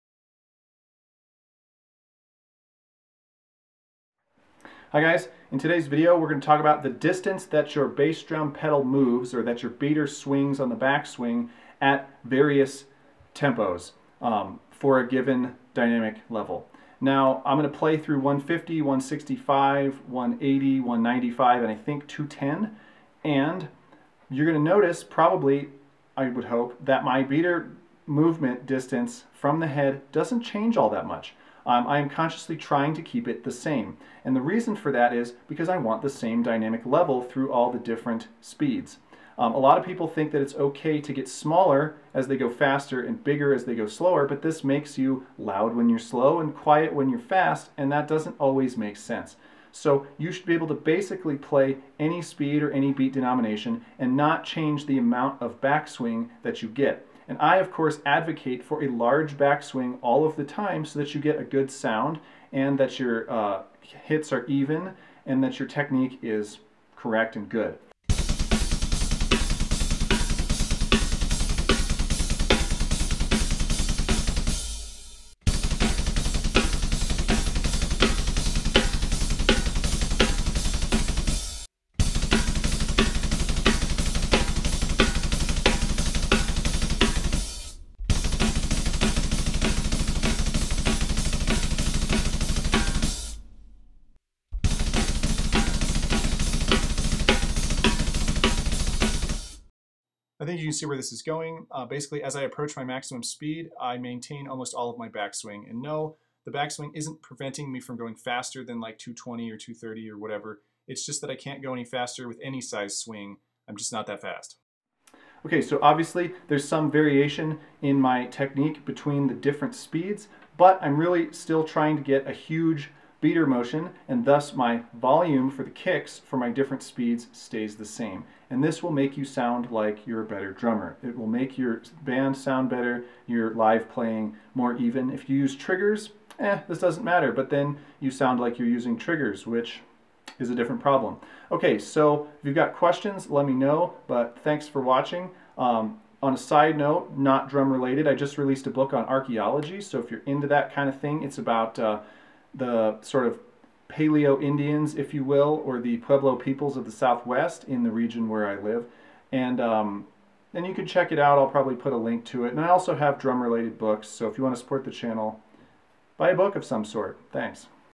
Kick Drum Pedal Motion - Beater Rebound and Speed
How big should your strokes be on kick drum when playing double bass? As big as they can be regardless of speed. That's the straight easy answer. Shoot for loud and large no matter the tempo or beat denomination and you'll sound better most of the time.

Archeology book, The Forgotten Side of Bandelier